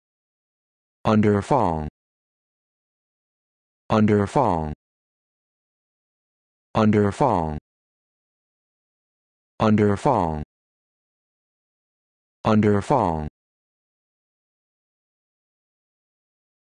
How to Pronounce Underfong
This video shows you how to pronounce Underfong